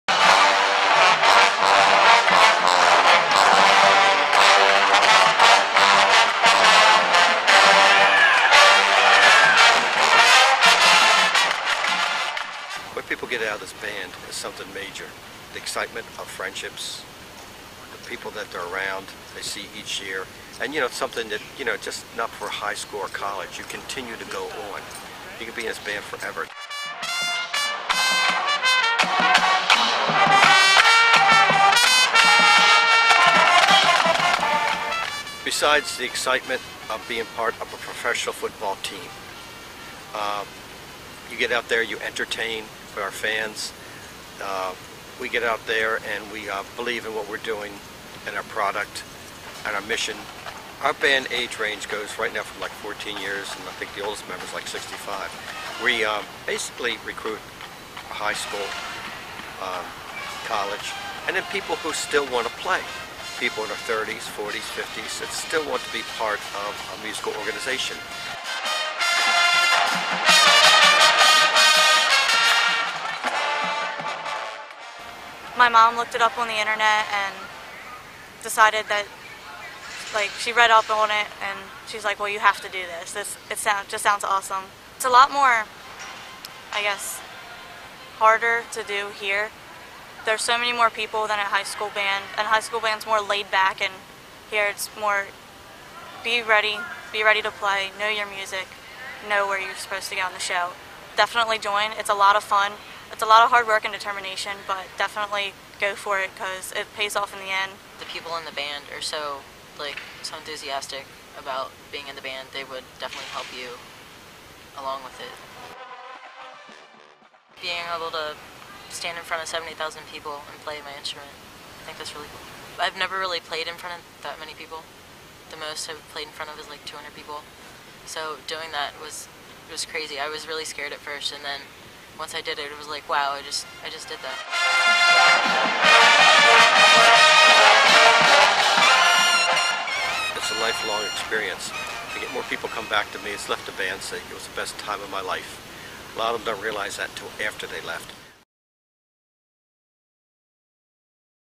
Kennard-Dale students march for Ravens
Kennard-Dale High School students Ben Meyer and Kaitltn Borig are part of the 2009 Baltimore Ravens' Marching Band.